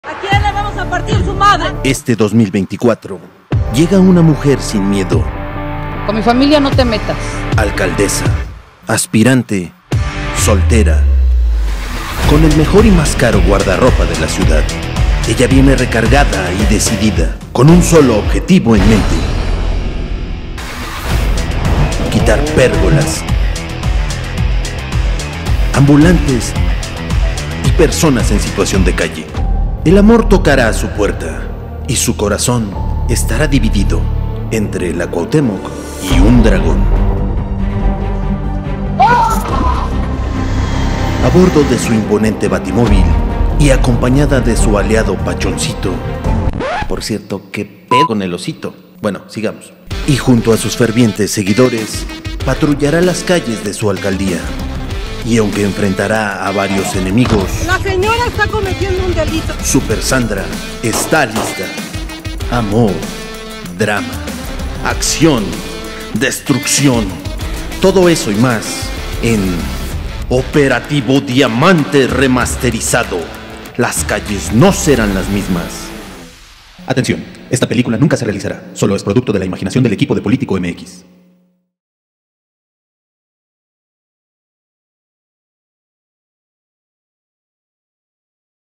Llega la 'nueva temporada' de Sandra Cuevas
La alcaldesa de Cuauhtémoc presentó su proyecto político, "Organización Por La Familia y La Seguridad De México" que busca convertir en partido político para 2025 y pelear en las próximas elecciones.